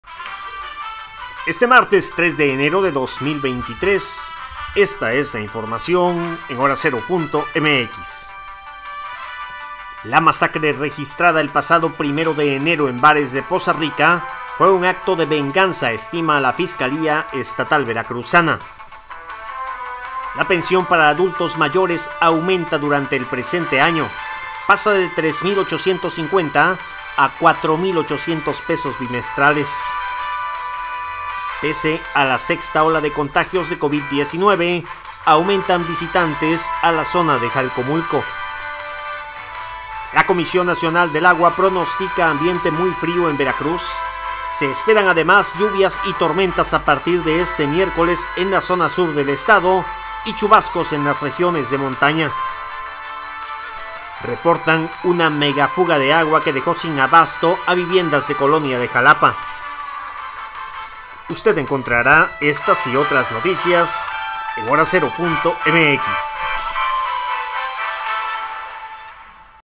Resumen de noticias Martes 3 de enero de 2023.
Este es el resumen de noticias de Hora cero Veracruz. Martes de 3 de enero de 2023.